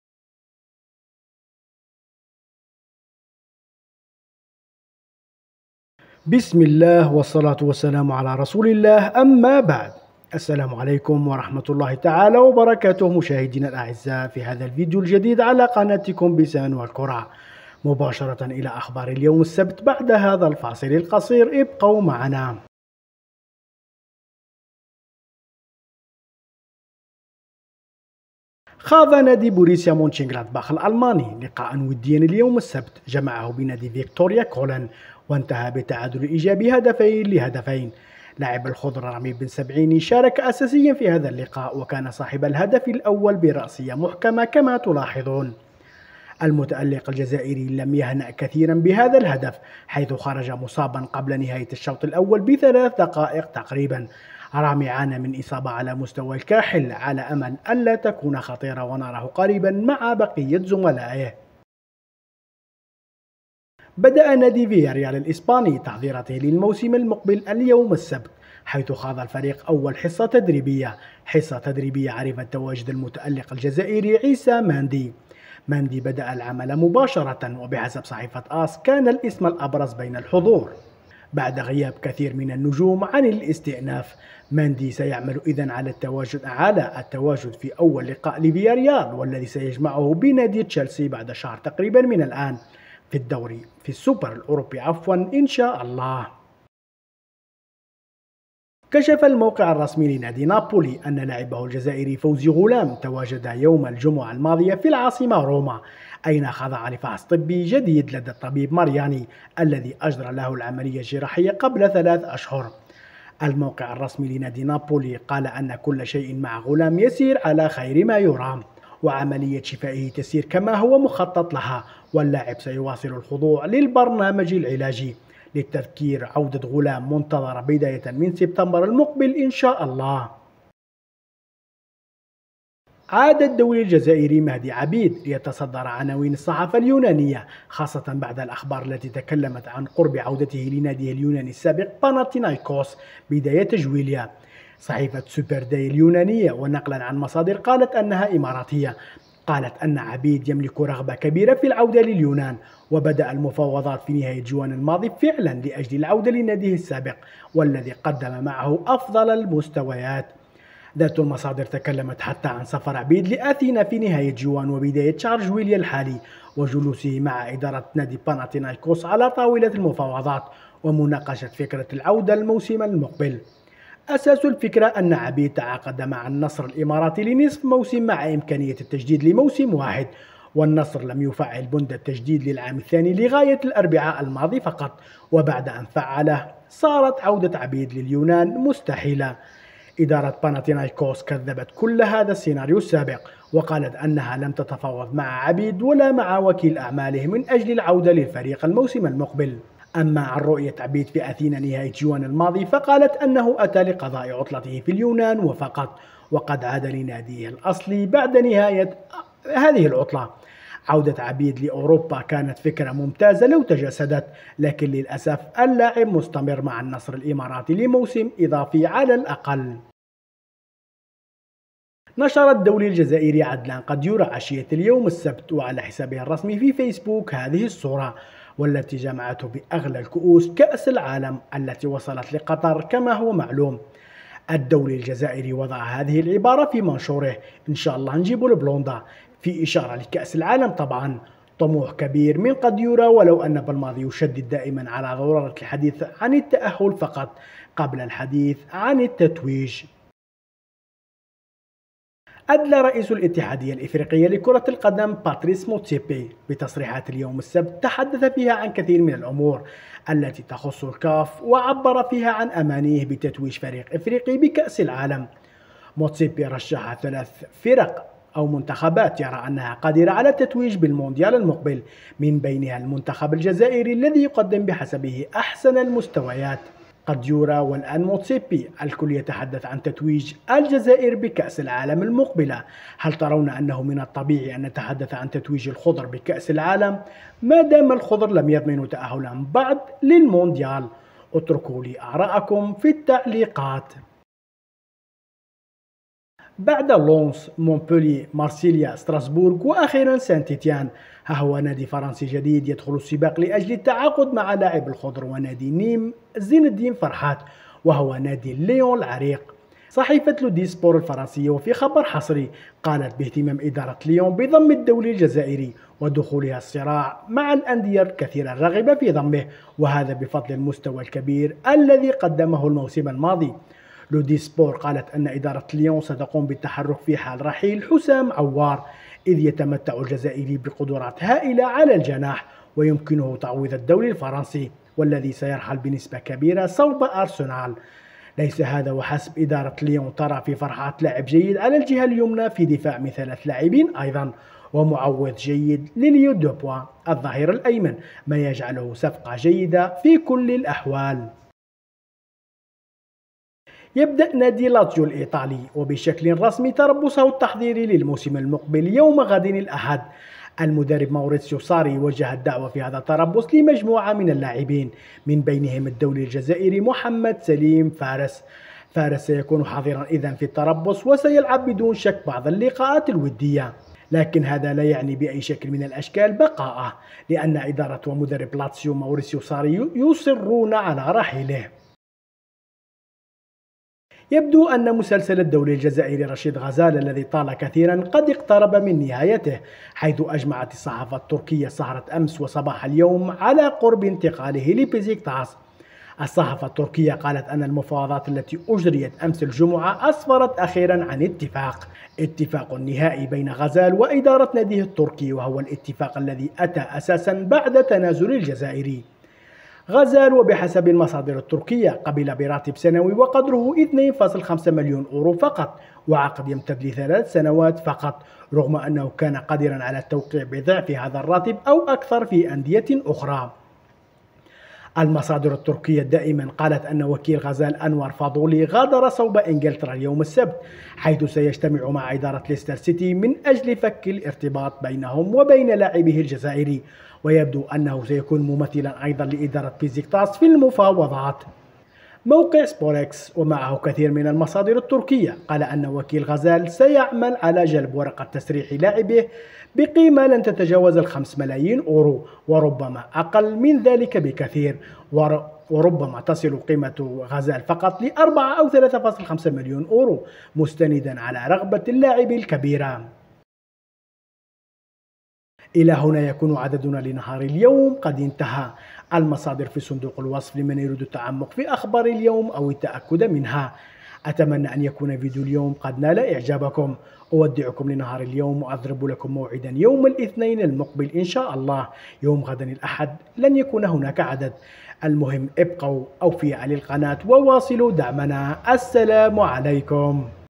هل ترشيح الجزائر للفوز بالمونديال منطقي؟عبيد اقترب من الرحيل لكن؟جديد غزال بن سبعيني ماندي غلام
المصادر

بن سبعيني

رئيس الكاف باتريس موتسيبي